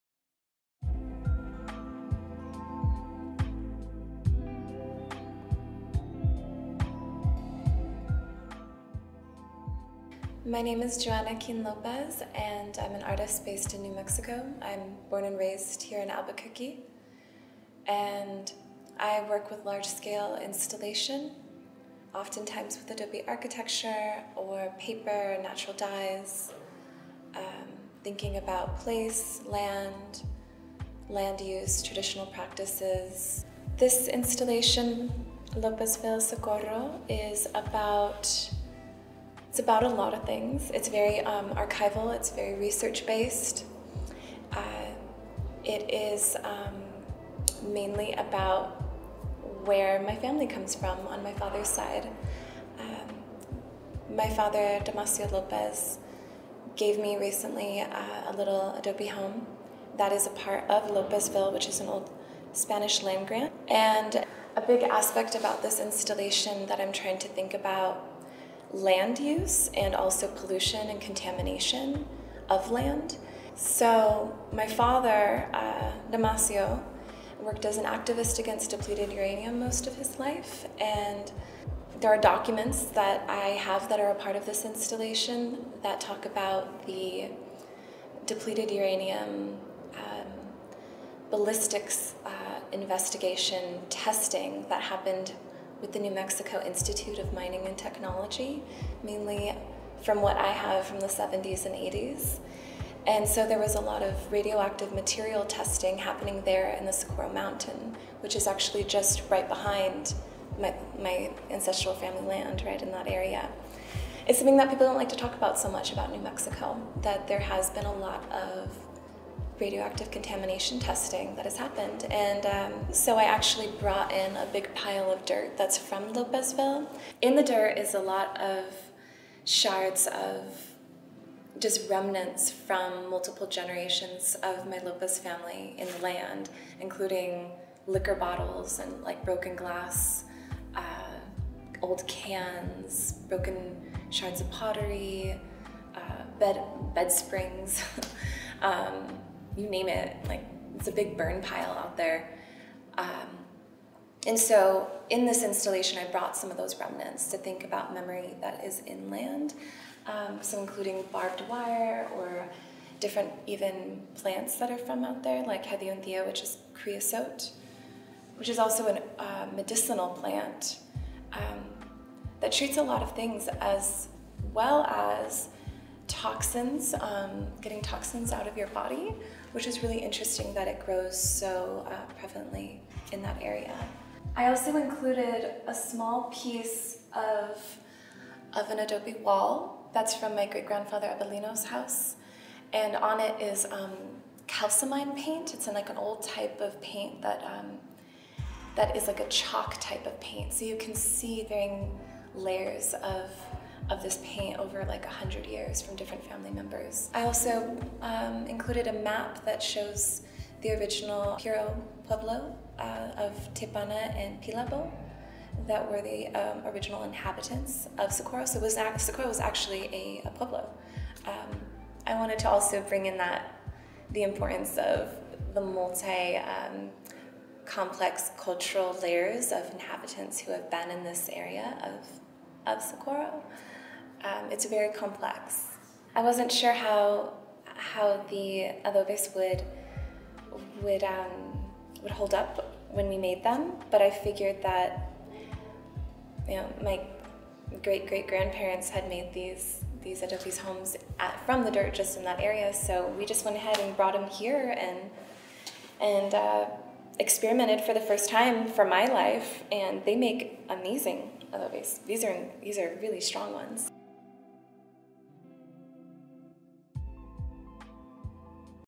Many Worlds Are Born Artist Talk - Joanna Keane Lopez
Joanna Keane Lopez is a multidisciplinary artist whose work blurs the boundaries between contemporary sculpture and architecture through the medium of adobe mud. By working with materials of adobe architecture, earthen plaster and alíz (a clay slip paint) her work acts to address conceptions of sculpture in engagement with land. Through the passing down of knowledge of the vernacular architecture techniques of the greater Southwest, Joanna creates work that seeks healing and reparation of fragmentation towards land, home, family and community that is connected to her own multi-generational roots in New Mexico. "Lópezville, Socorro" is a manta de techo, the dried stems of a chaparral or creosote bush, and at least four generations of remnants—glass bits, barbed wire, and old cans—evidence of past generations of Lopez family members who made their lives on the same sprawling plot of land.

Interview conducted, filmed and edited by Daniel Ulibarri